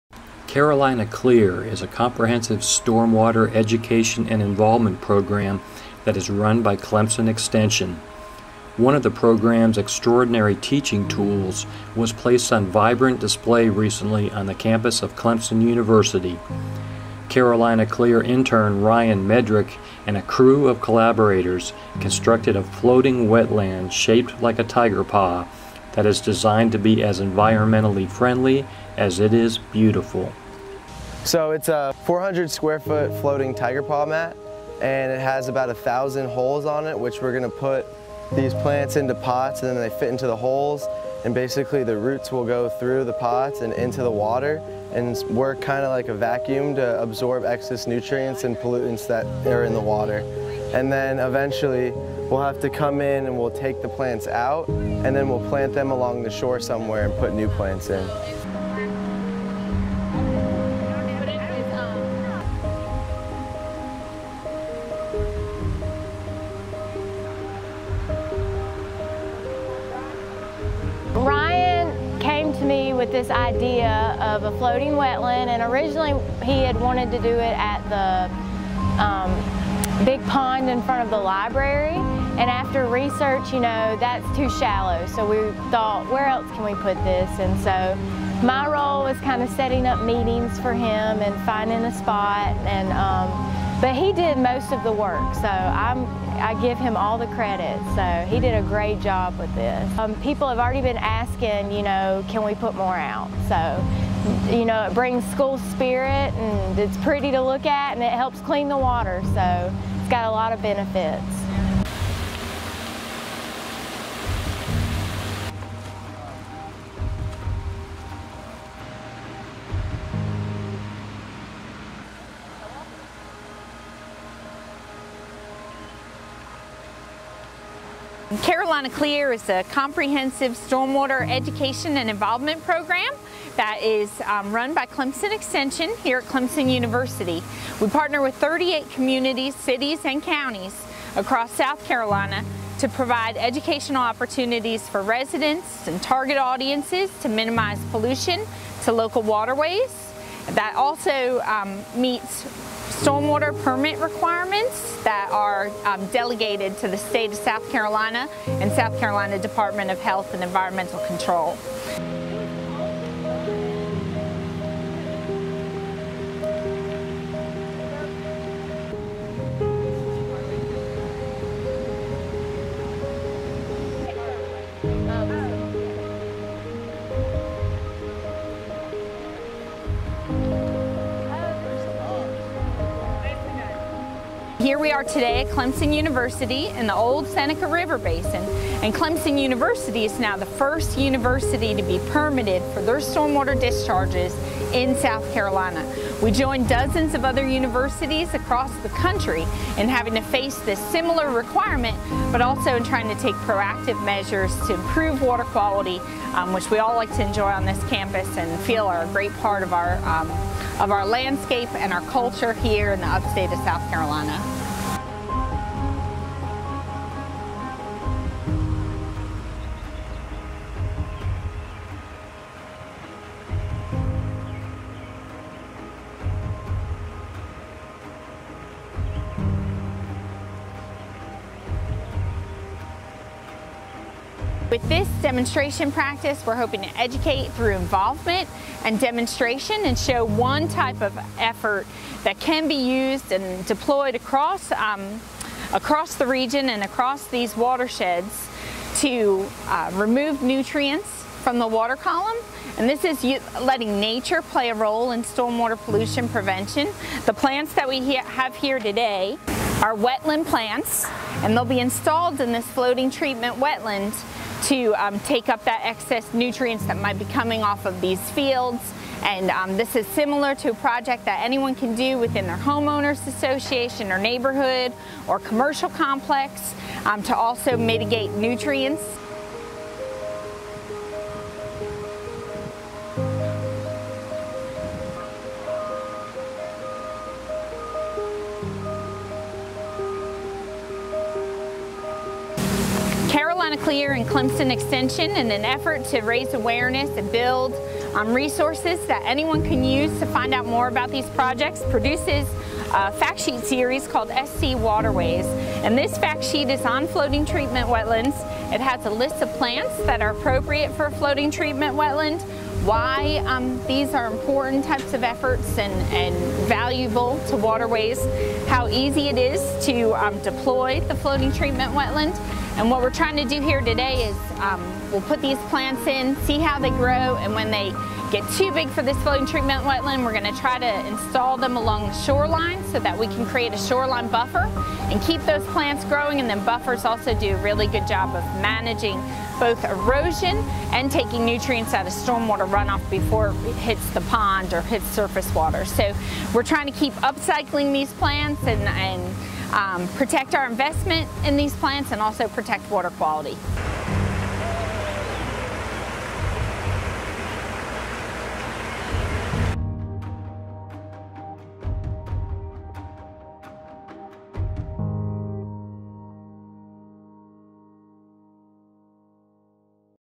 Floating treatment wetland
Carolina Clear is a comprehensive stormwater education and involvement program that is run by Clemson Extension. One of the program’s extraordinary teaching tools was placed on vibrant display recently on the campus of Clemson University. Carolina Clear intern Ryan Medric and a crew of collaborators constructed a floating wetland shaped like a Tiger paw that is designed to be as environmentally friendly as it is beautiful.